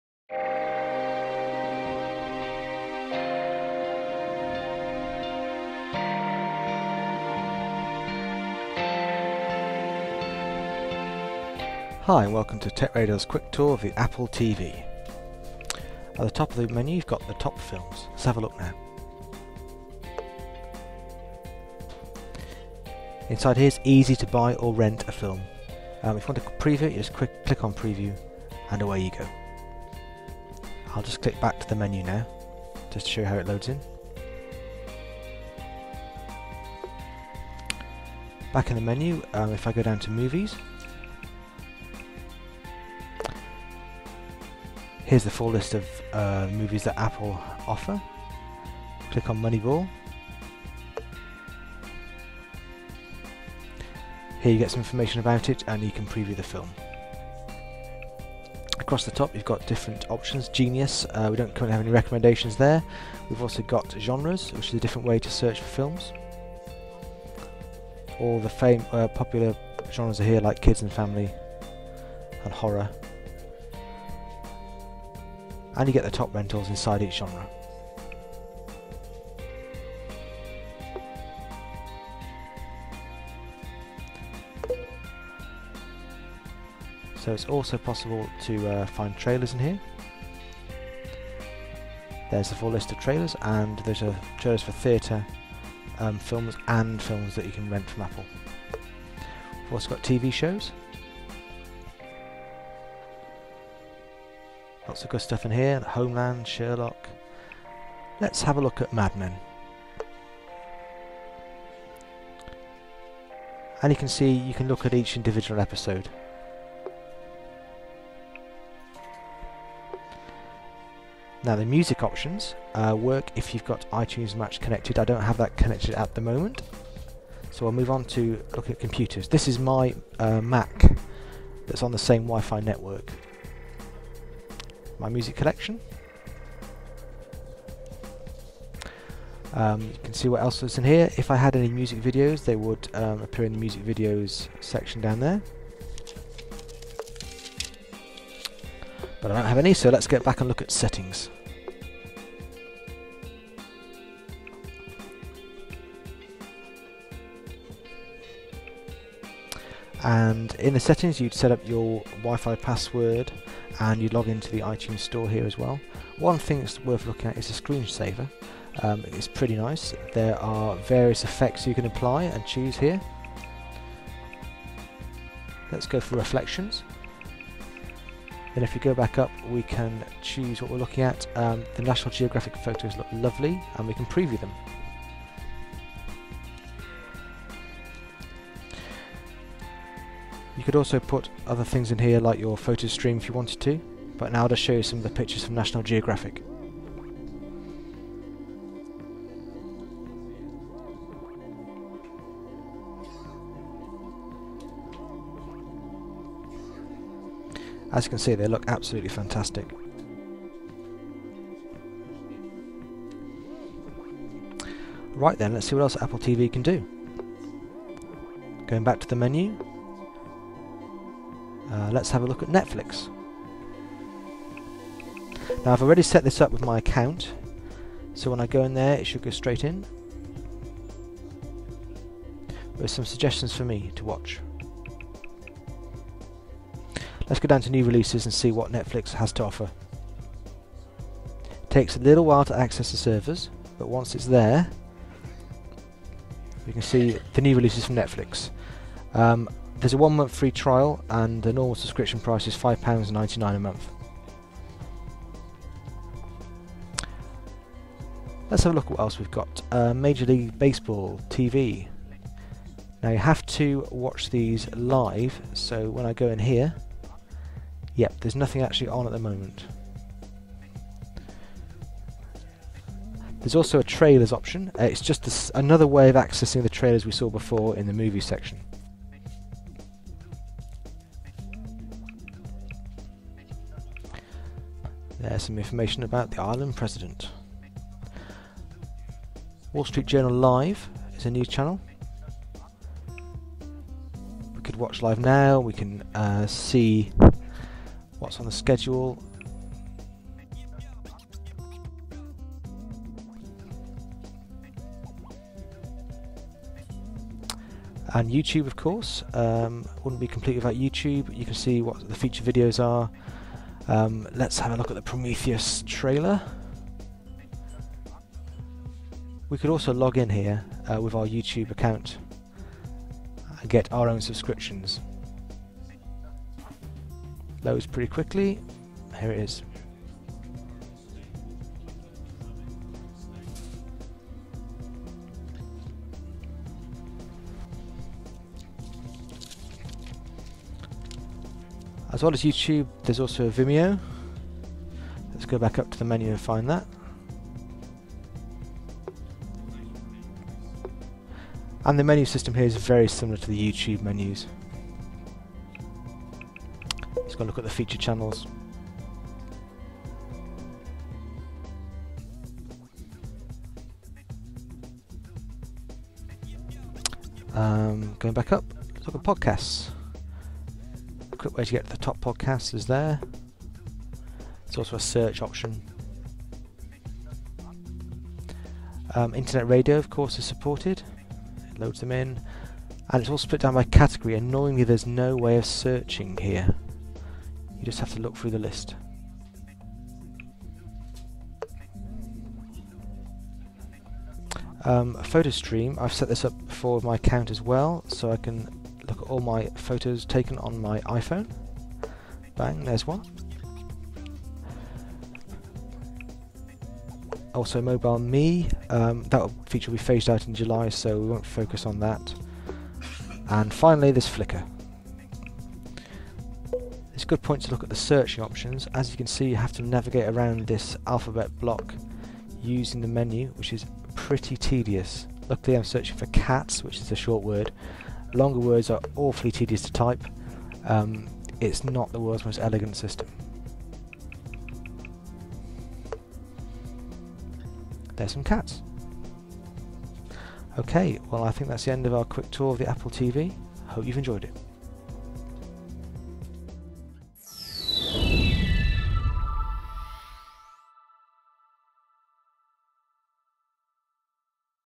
Apple TV walkthrough Video
A step-by-step guide to the new Apple TV interface